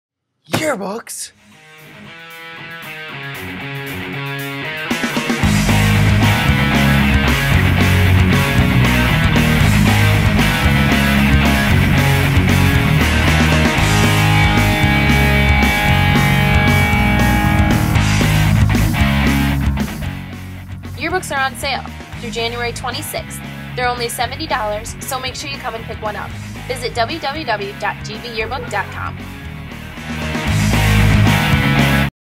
Grand Blanc High School Yearbooks For Sale
The Echo 2011 Grand Blanc High School Yearbook Staff encourages all students to purchase a yearbook by Wednesday January 26th for only $70. The price of yearbooks will increase after this deadline and availability is not guaranteed.

We Are Greater!!